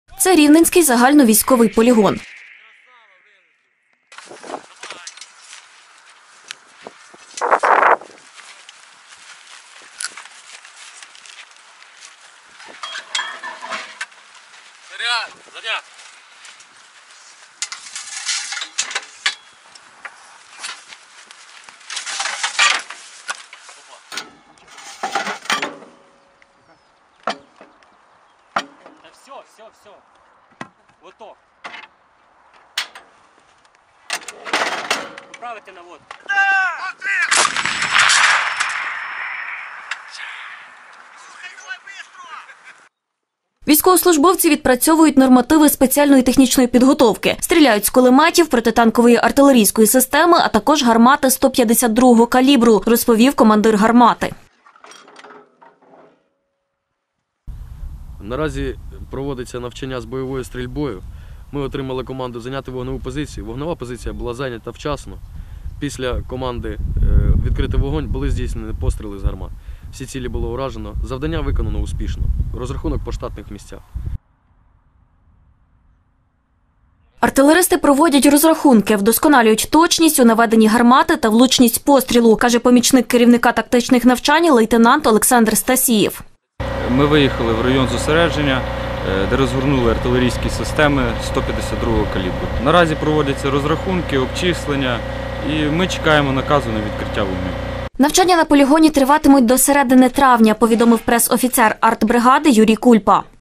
Тернопільські артилеристи беруть участь у навчаннях на Рівненському полігоні
Військовослужбовці Тернопільської артилерійської бригади проходять навчання з бойової підготовки на Рівненському військовому полігоні. Це планові навчання – повідомив пресофіцер Тернопільської артбригади Юрій Кульпа.